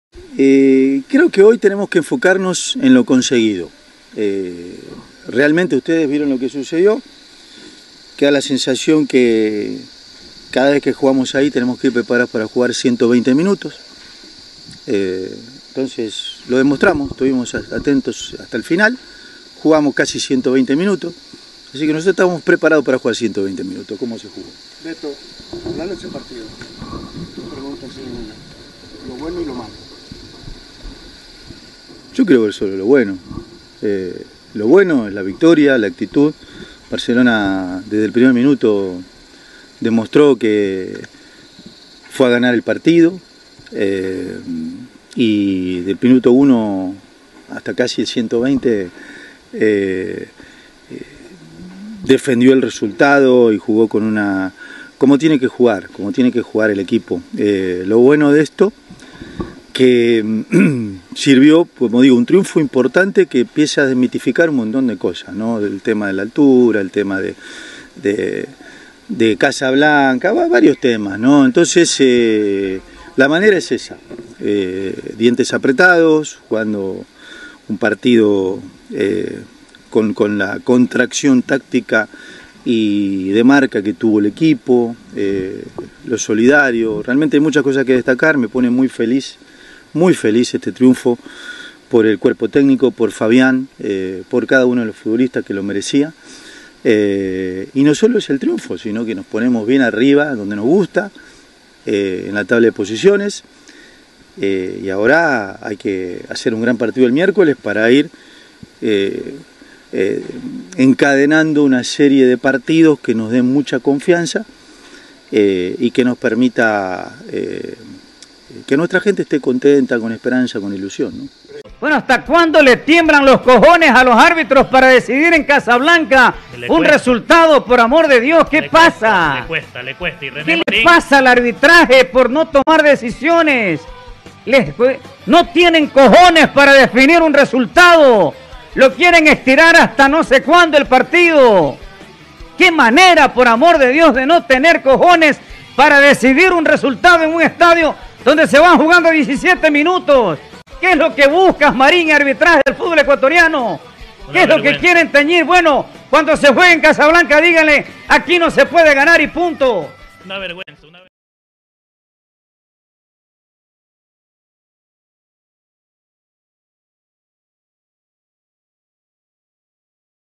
LIGA DE QUITO VS BARCELONA SC 0 - 1 | ALFARO EN CASA BLANCA JUGAMOS 120 MINUTOS
APOYANOS CON TU DONACION DE 1 DOLAR DANDO CLIC EN EL BOTON UNIRSE